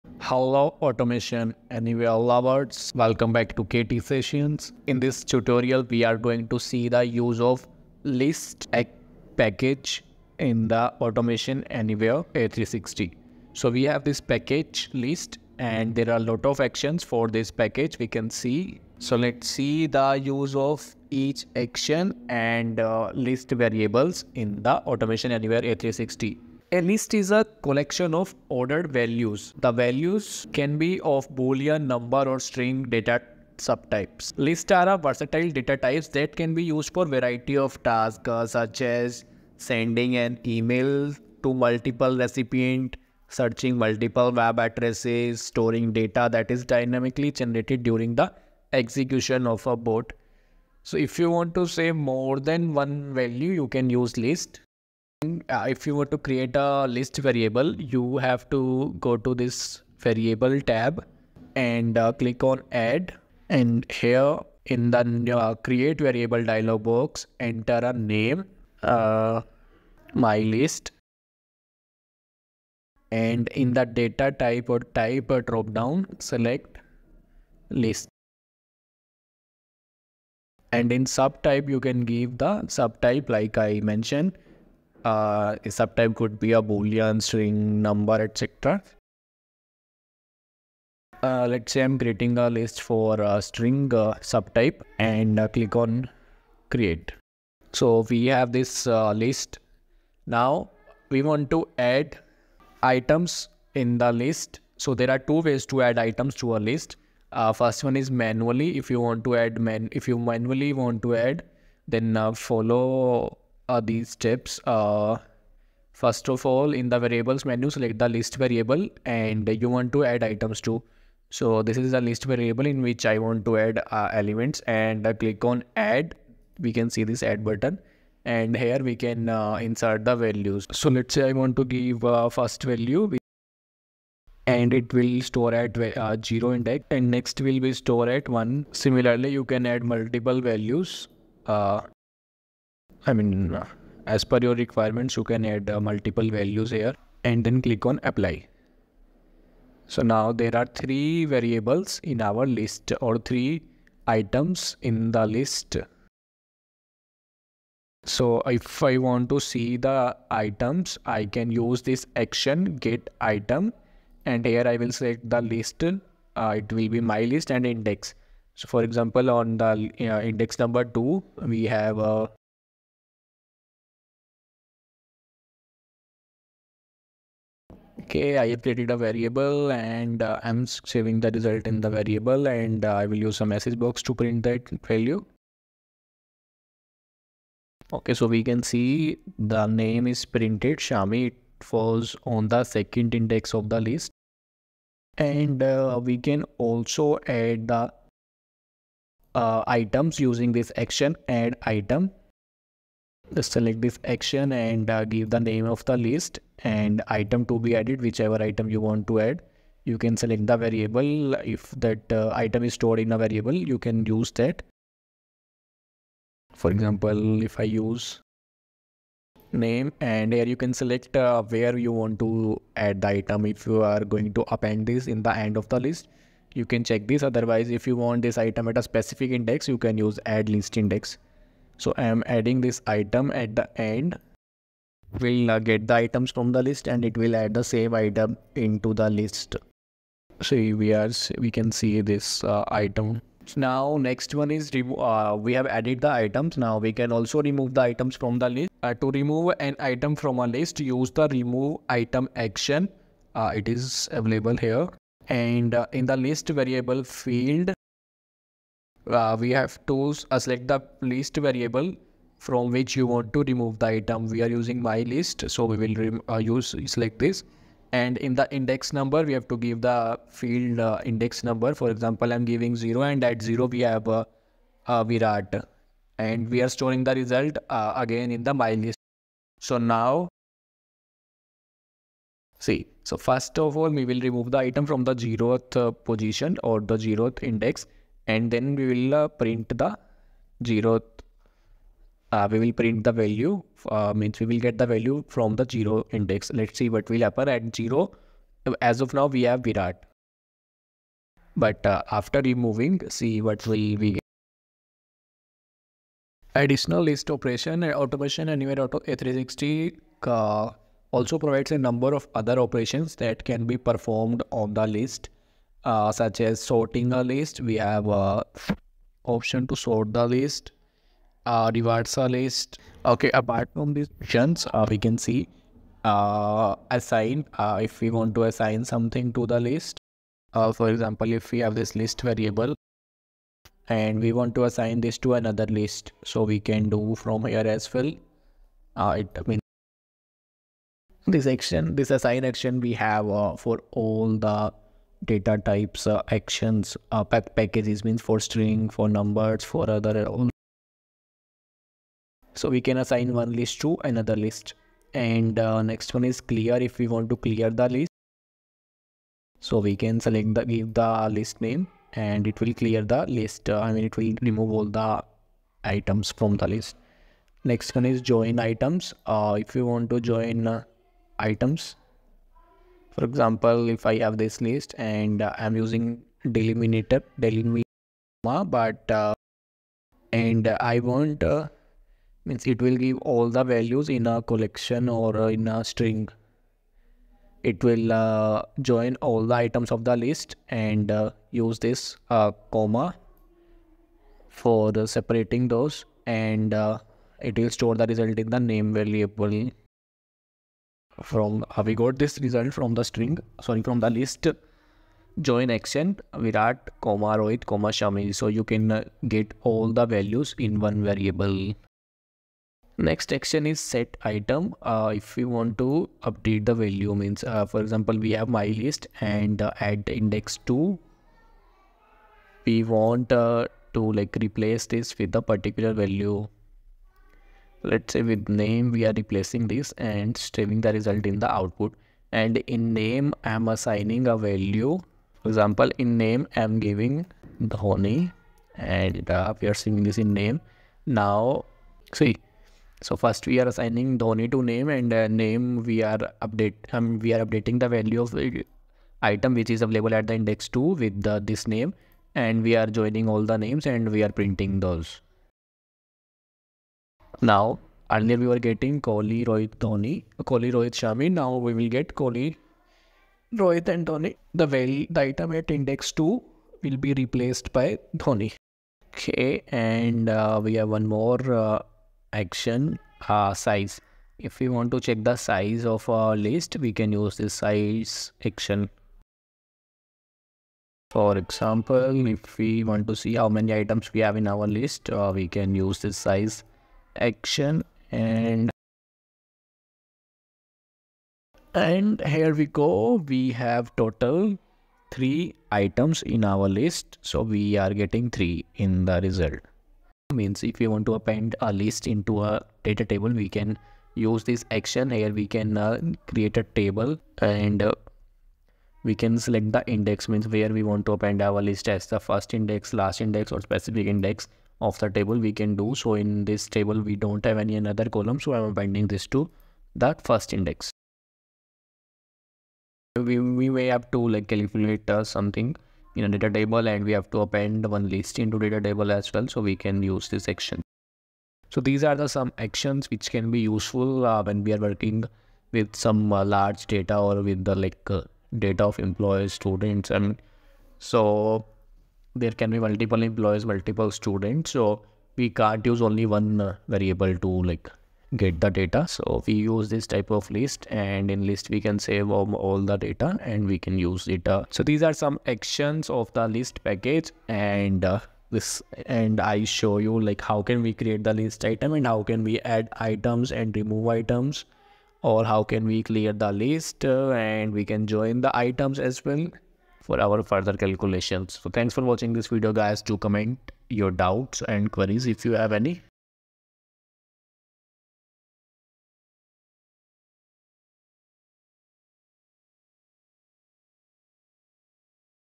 Automation Anywhere A360 - List Package | List operations in A360 | KT Sessions
Unlocking the Secrets of List Package in Automation Anywhere A360

Description:
🔍 🔵 Dive into the world of automation with Automation Anywhere! 🚀 In this video, discover the power of List package in the Automation Anywhere tool. We unravel its actions, showcase practical applications, and guide you step-by-step through implementation. Elevate your automation game—watch now! 🤖✨

🎥 Timestamps:
0:00 - Introduction
0:56 - Create List Variable
3:45 - Add items in list
6:12 - Remove items in list

📌 Topics Covered:
🚀 Create List Variable
🔬 Add/Remove List Variable
📈 List Operations
🤔 Others

🛍️ Featured Products used in video:
🔵 Dell Wireless Keyboard/Mouse
🔧 OnePlus Nord CE 3 5G
🌐 Apple 2022 iPad Air M1 Chip

What topics do you want us to cover next? Let us know!